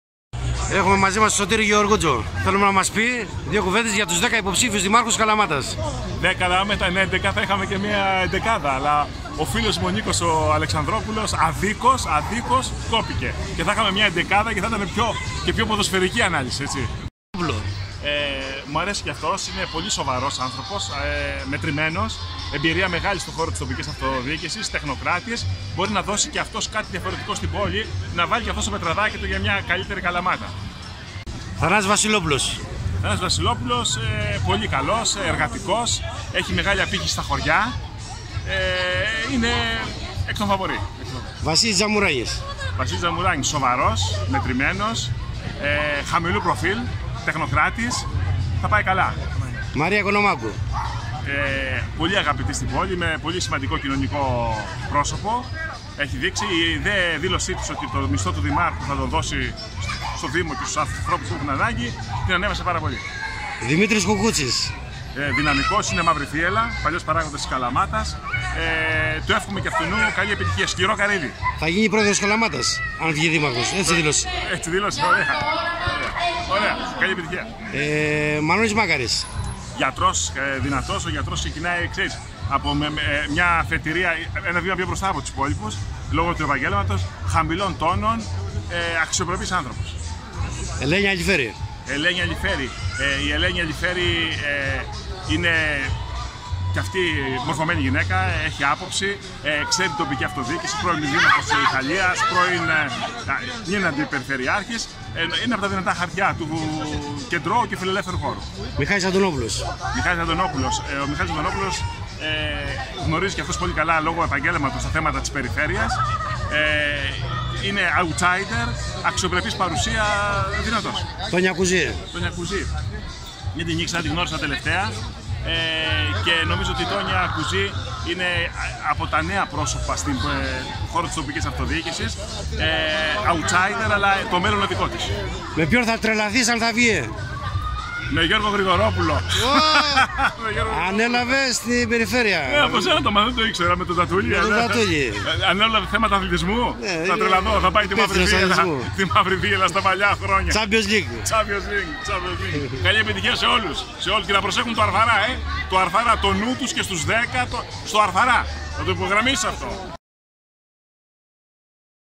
Σωτήρης Γεωργούντζος θα «τρελαθώ» αν βγει δήμαρχος Καλαμάτας….
Ο owner του kalamatatimes.gr Γιάννης Μαλαπέτσας, συνάντησε τον γνωστό σε όλη την Ελλάδα Σωτήρη Γεωργούντζο για τον ιδιαίτερο τρόπο σχολιασμού του στους ποδοσφαιρικούς αγώνες της Μαύρης Θύελλας και δεν θα μπορούσε να μην του ζητήσει να περιγράψει τους 10 υποψήφιους δημάρχους της Καλαμάτας